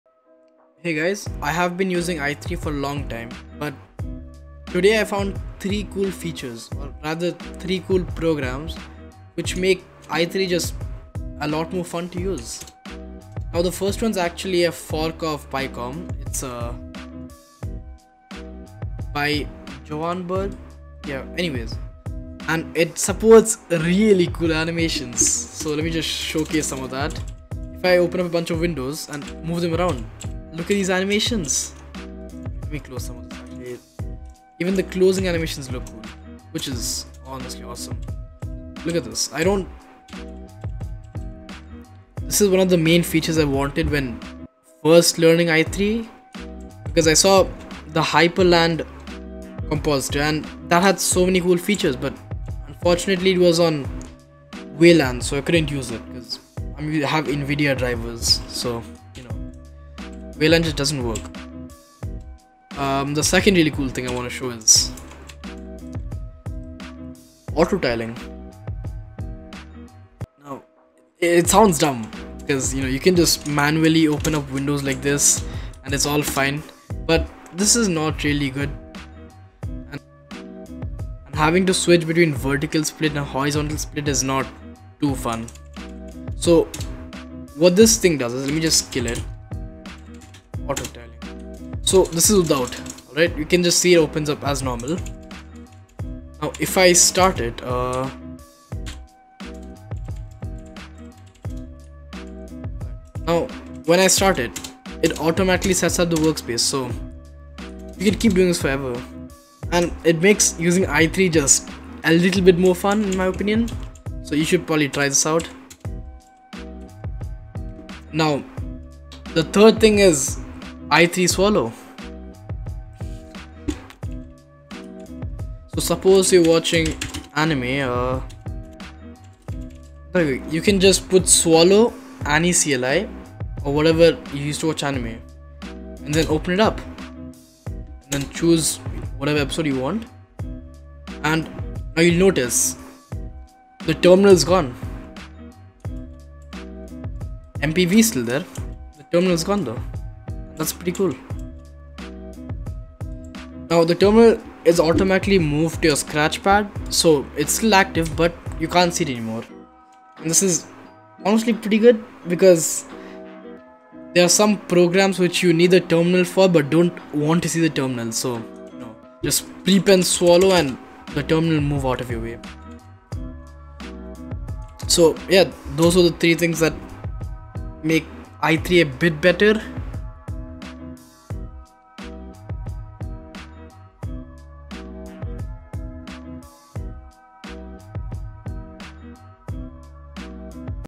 These 3 programs will change how you use I3-wm
I stumbled across 3 programs that either made using i3 more pleasant or made it look way better

The programs showcased in the video are as follows:-

Below are tags , ignore them